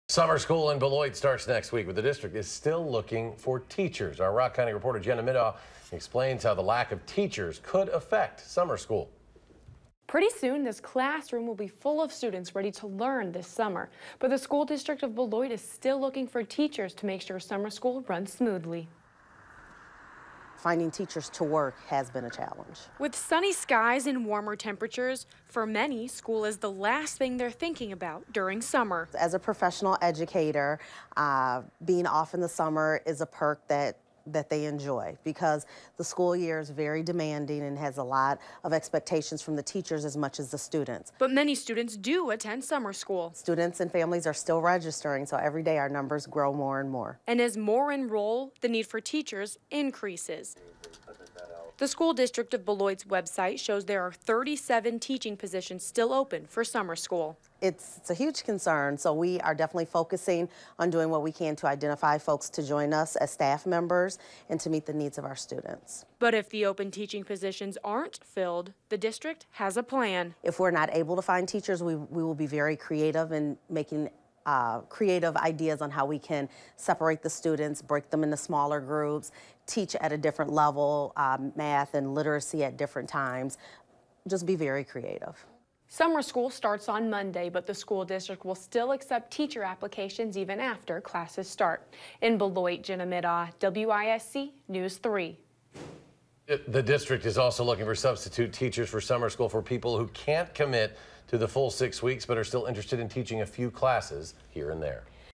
Beloit School District looking for more summer school teachers
The School District of Beloit is trying to fill a few dozen teaching positions before summer school starts Monday.
Jenna Middaugh reports.